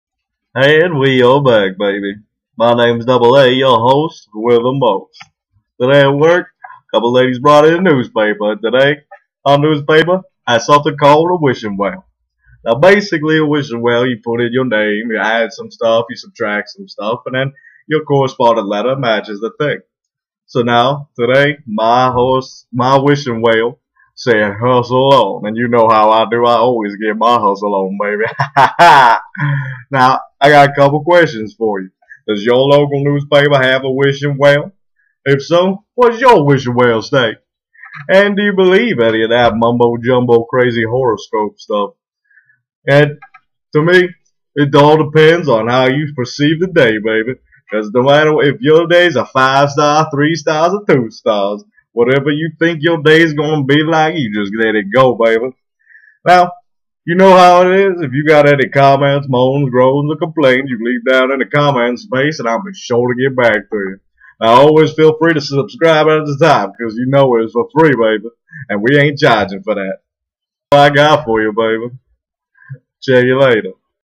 WISHING WELL
Hustle On Baby! Our local newspaper has a wishing well.  Double A gets his Hustle On and tells you a little about it.